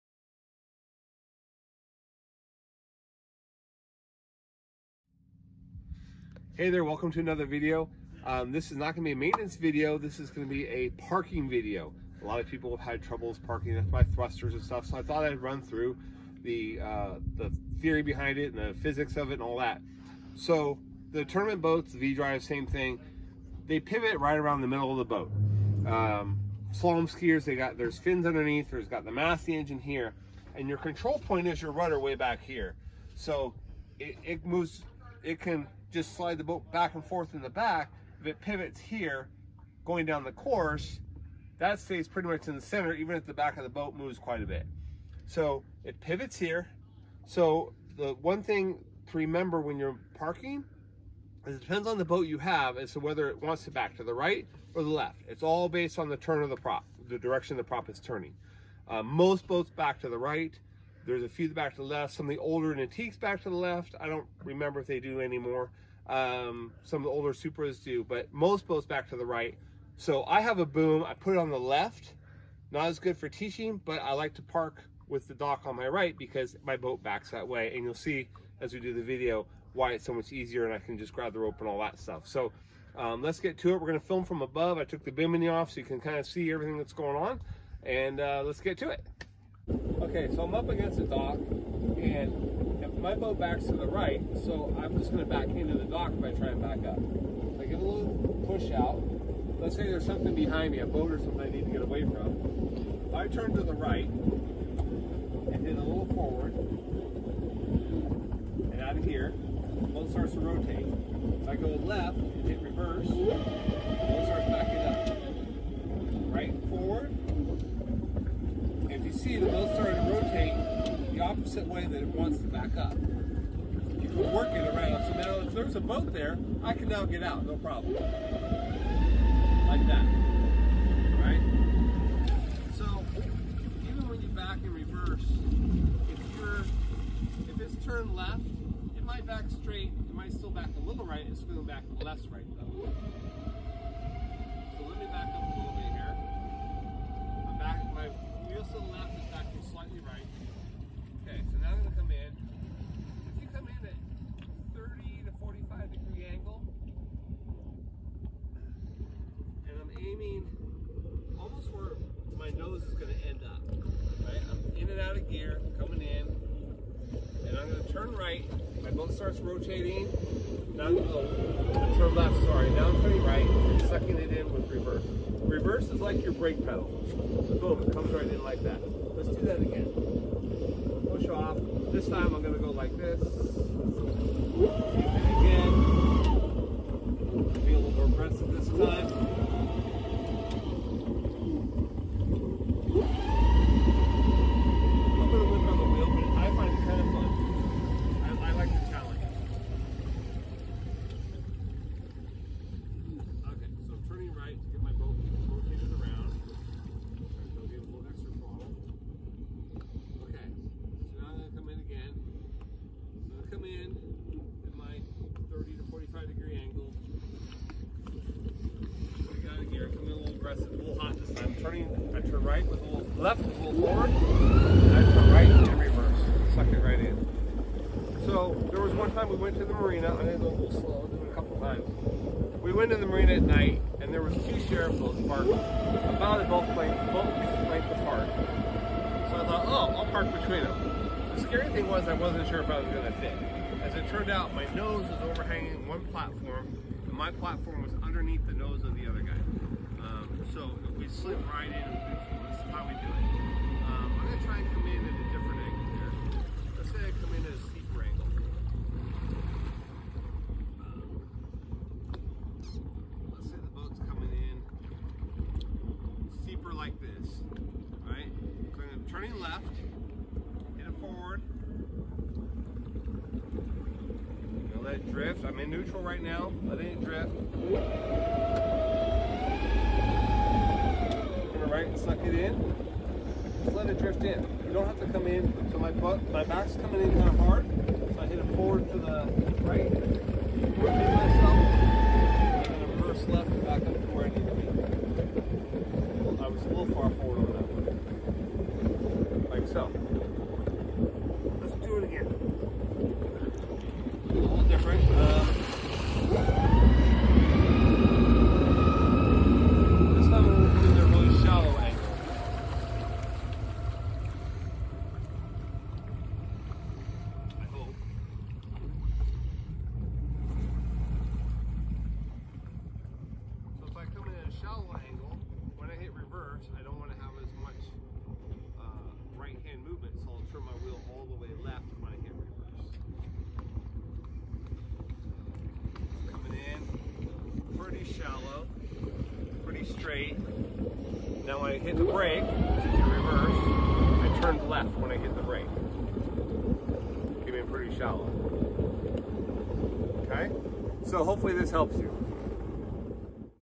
Docking an Inboard
This video is an aid to teach you the theory behind docking an inboard boat.